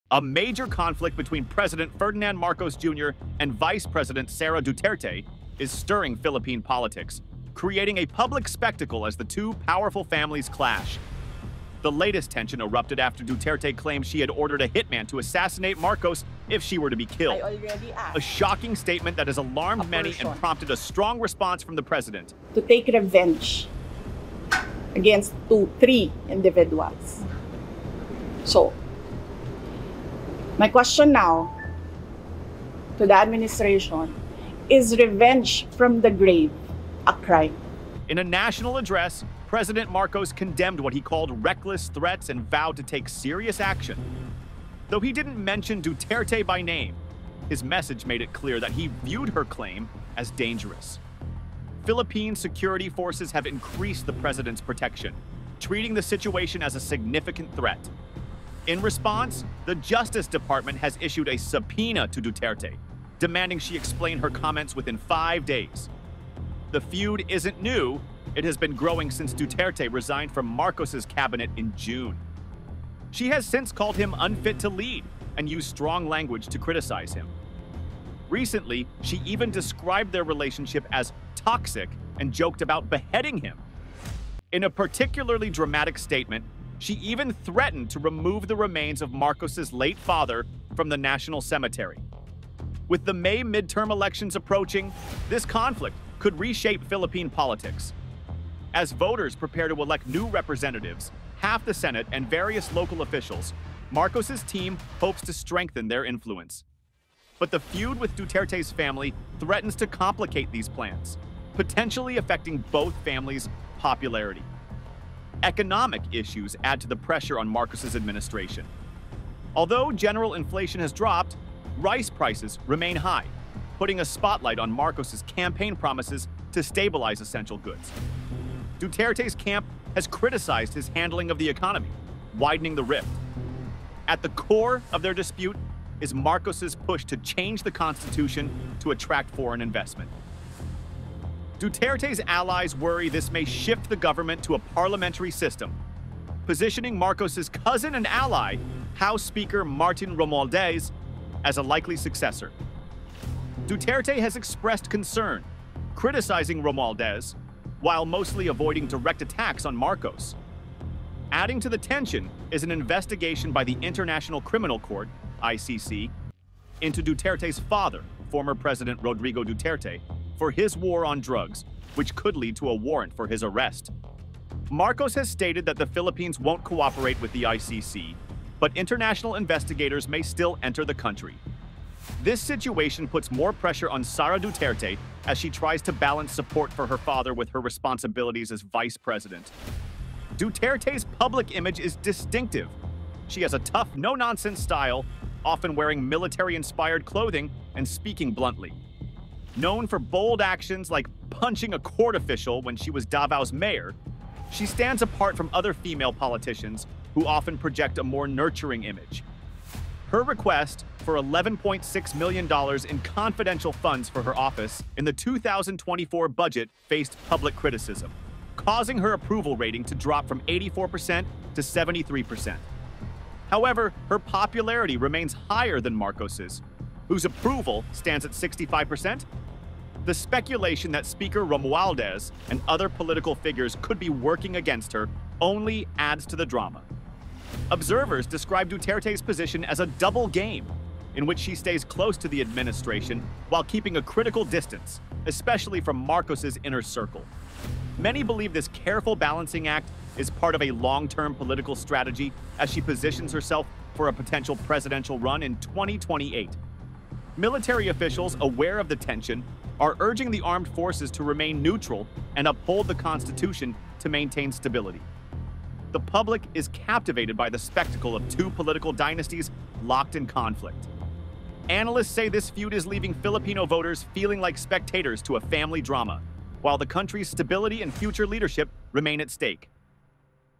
VP Duterte Threatens to Assassinate President Marcos Jr.
A fierce conflict between Philippine President Ferdinand Marcos Jr. and Vice President Sara Duterte is dominating the political landscape, escalating into a public feud between two of the nation’s most powerful families. The latest flashpoint came when Duterte claimed she had ordered a hitman to assassinate Marcos if she were killed, prompting the president to condemn the statement as reckless and a national security threat. Tensions have been simmering since Duterte resigned from Marcos’s cabinet in June, accusing him of being unfit to lead and criticizing his push for constitutional reforms that could benefit his ally, Speaker Martin Romualdez. This rift, compounded by high rice prices and criticism of Marcos’s economic policies, threatens to shape the upcoming midterm elections as both families vie for public favor. Meanwhile, Duterte’s approval rating, though higher than Marcos’s, has dipped following her controversial $11.6 million confidential funds request. Adding to the drama, the International Criminal Court’s investigation into her father, Rodrigo Duterte, intensifies the stakes. Analysts describe Sara Duterte’s approach as a strategic balancing act, maintaining distance from Marcos while positioning herself for a potential 2028 presidential bid. With the military urging neutrality and voters engrossed in the dynastic clash, this political rivalry underscores deep divisions and raises concerns about the nation’s stability and future leadership.

0:00 Conflict between President Ferdinand Marcos Jr. and Vice President Sara Duterte
1:06 The feud isn’t new
1:54 Economic issues add to the pressure on Marcos’s administration
2:14 At the core of their dispute is Marcos’s push to change the constitution
2:40 An investigation by the International Criminal Court (ICC) into Duterte’s father
3:09 Duterte’s public image is distinctive
3:58 Observers describe Duterte’s position as a “double game”